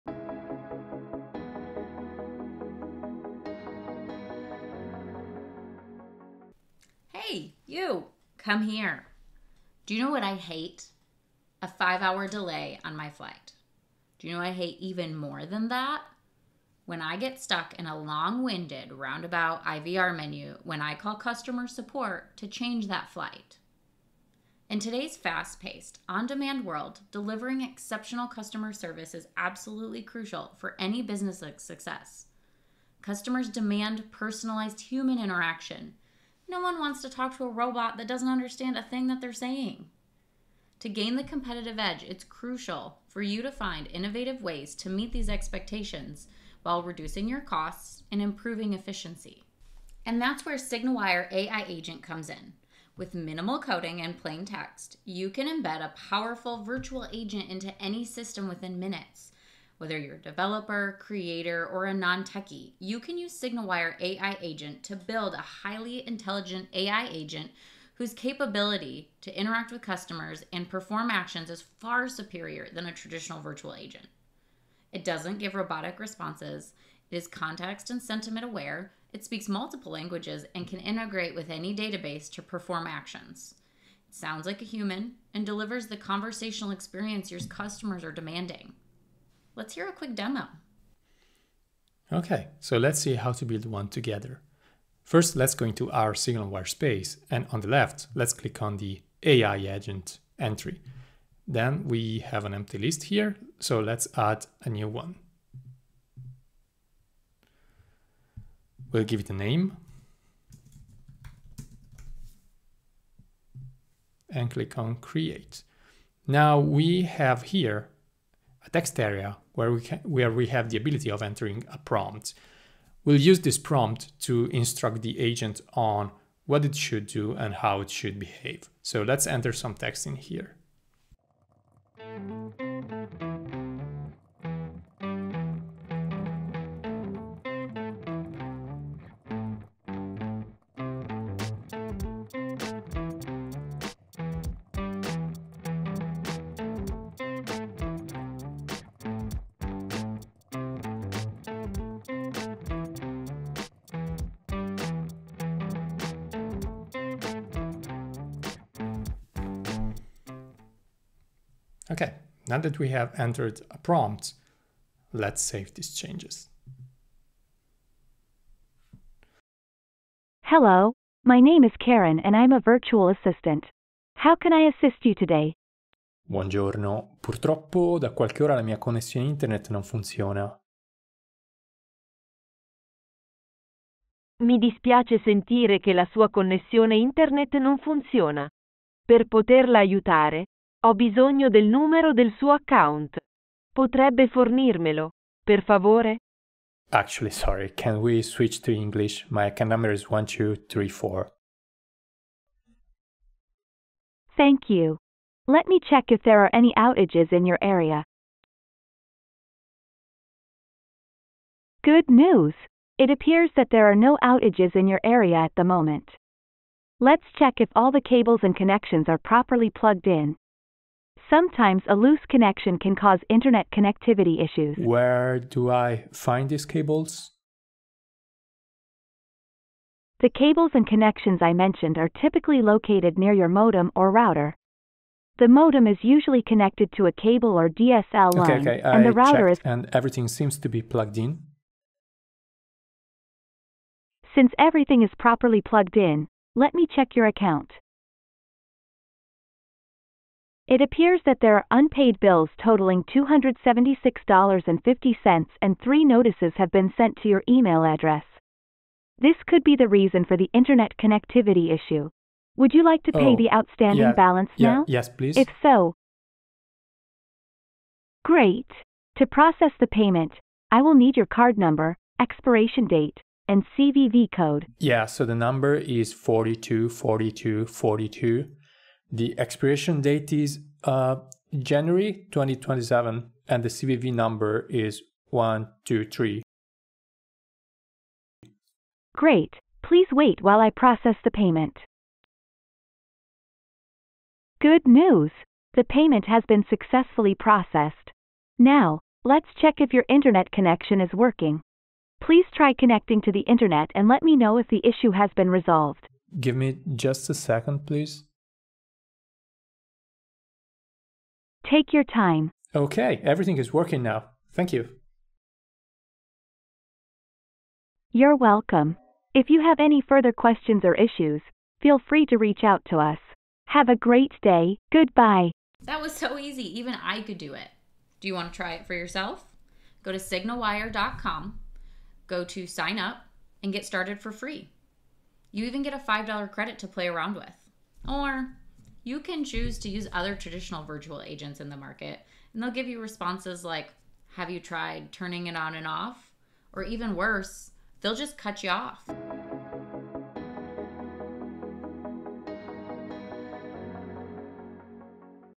Our new AI Agent speaks to you like a human and fixes your internet
With SignalWire's new AI agent, you can create complex artificial intelligence voice bots that can switch languages, search account information, and troubleshoot basic support issues. Oh - and it can all be built using no-code tools. Don't worry, you can still use lots of code if that's your style.